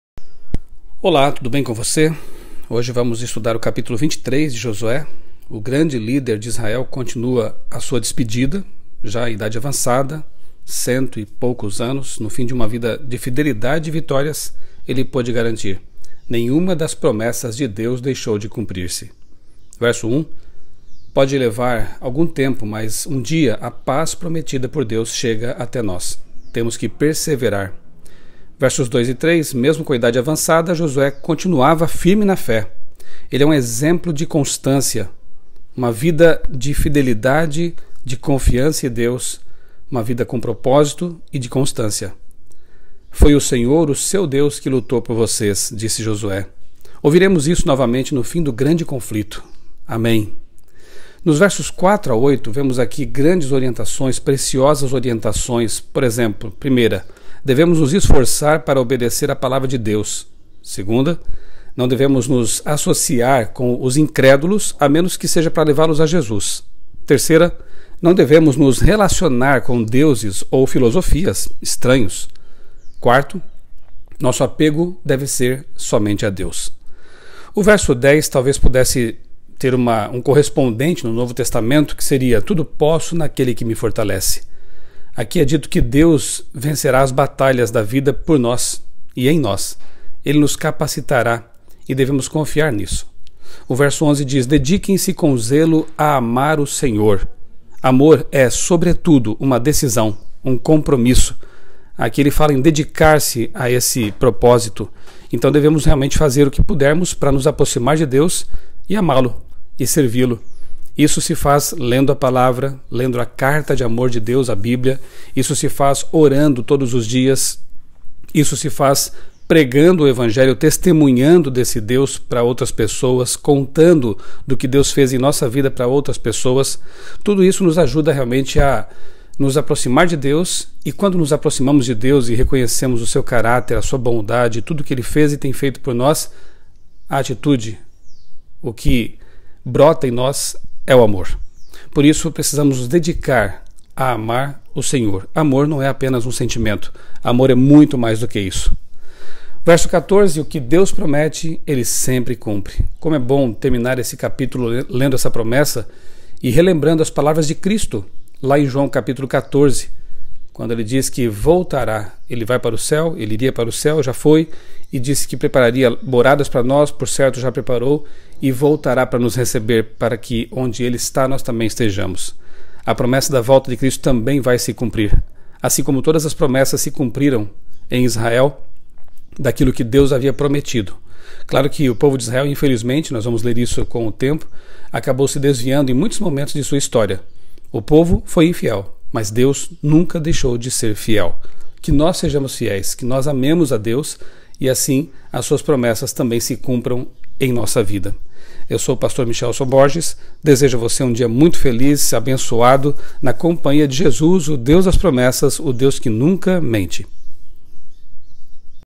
Josué 23 - Pastor Michelson Borges
Js 23 – No fim de uma vida de fidelidade e vitórias, Josué pôde garantir: “Nenhuma das promessas [de Deus] deixou de cumprir-se.”

Js 23:1 – Pode levar algum tempo, mas um dia a paz prometida por Deus chega até nós. Temos que perseverar.

Js 23:2, 3 – Mesmo com idade avançada, Josué continuava firme na fé. Que exemplo da constância!

Js 23:3 – “Foi o Senhor, o seu Deus, que lutou por vocês.” Ouviremos isso novamente no fim do grande conflito. Amém!

Js 23:4-8 – Versos com orientações preciosas: (1) devemos nos esforçar para obedecer à Palavra de Deus; (2) não devemos nos associar com os incrédulos, a menos que seja para levá-los a Jesus; (3) não devemos nos relacionar com deuses (filosofias) estranhos; (4) nosso apego deve ser somente a Deus.

Js 23:10 – Versão Novo Testamento: “Tudo posso nAquele que Me fortalece.”

Js 23:11 – “Dediquem-se com zelo a amar o Senhor.” Amor é, sobretudo, uma decisão, um compromisso.

Js 23:14 – O que Deus promete Ele sempre cumpre.

Pastor Michelson Borges

Pastor  Valdeci Junior